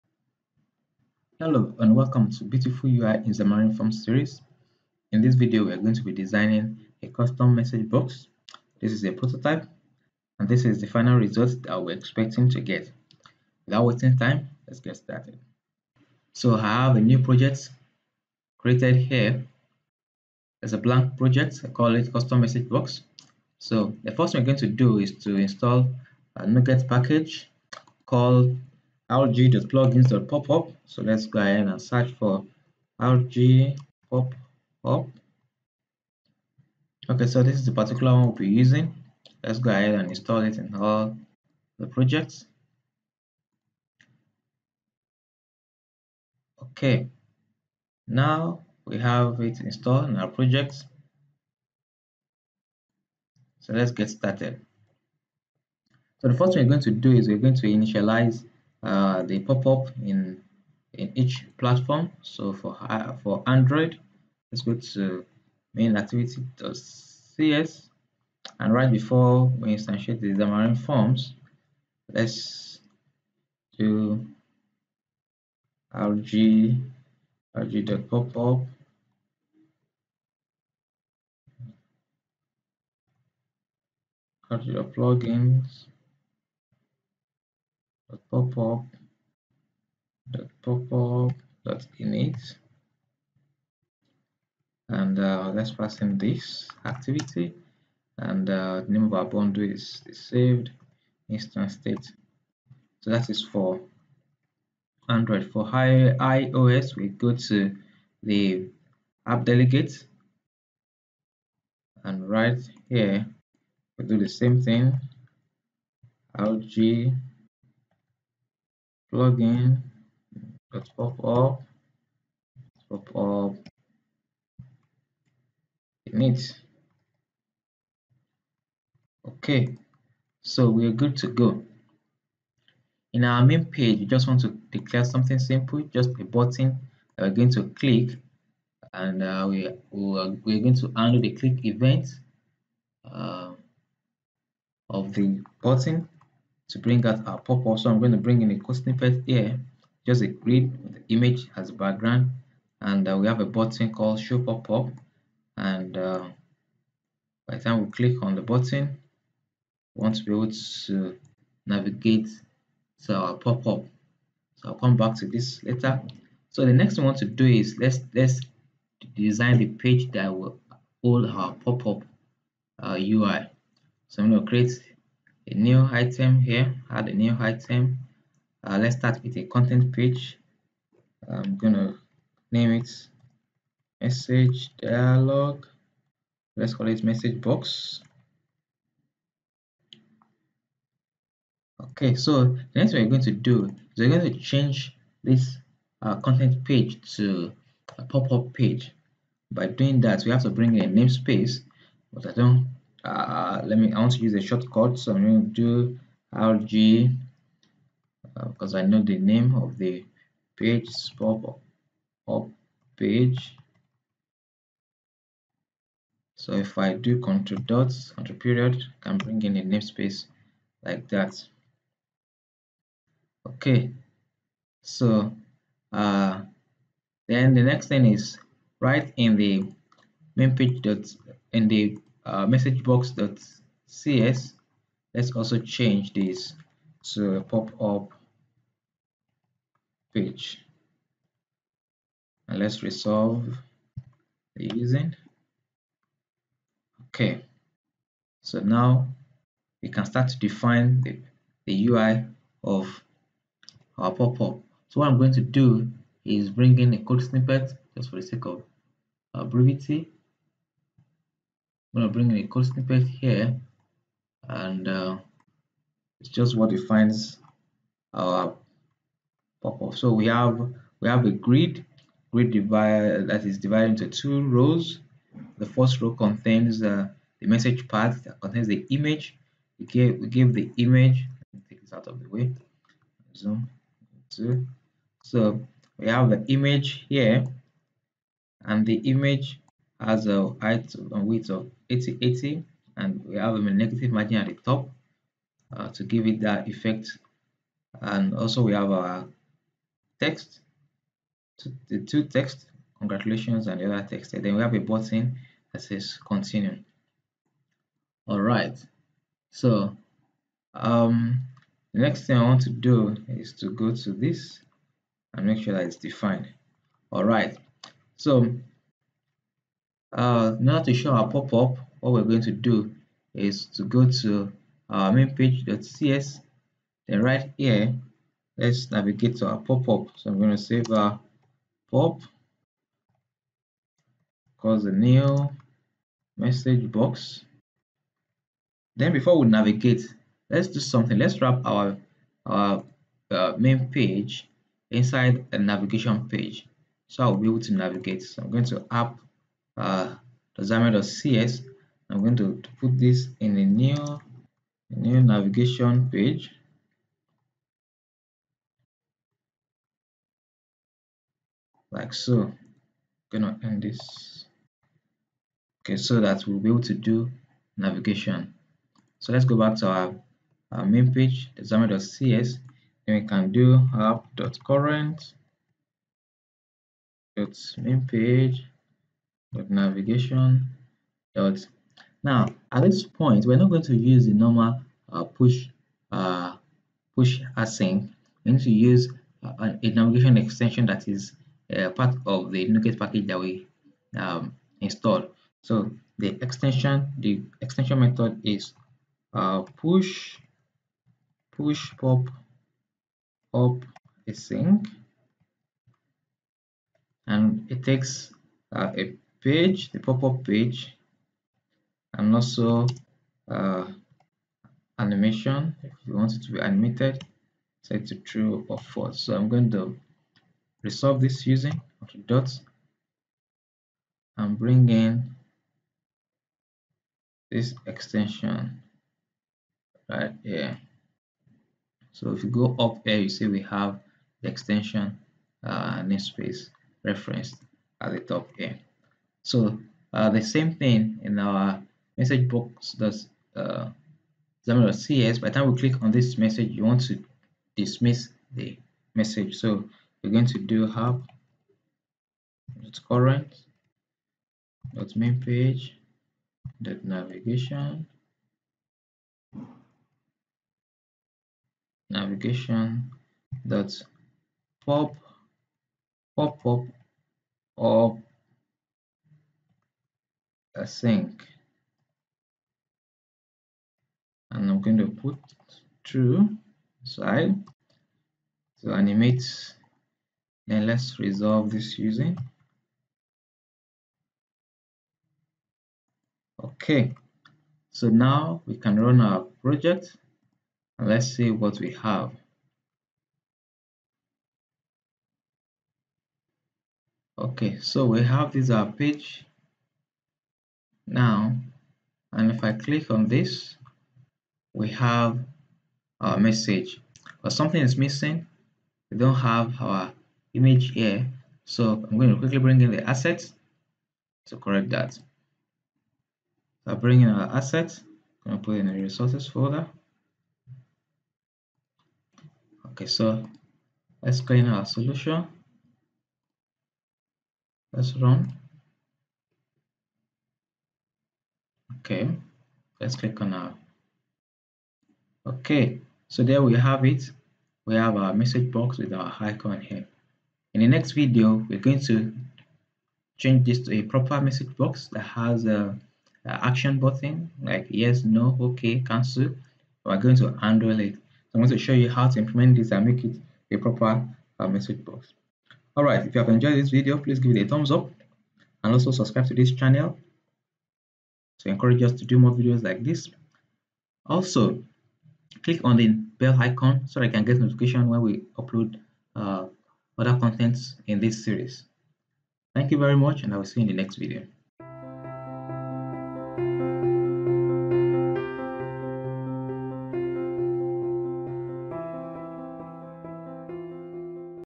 Custom Message Box in Xamarin Forms (Part 1)
In this video, you will learn how to design a custom message box using a transparent popup.

➤ My Gadgets
USB Hub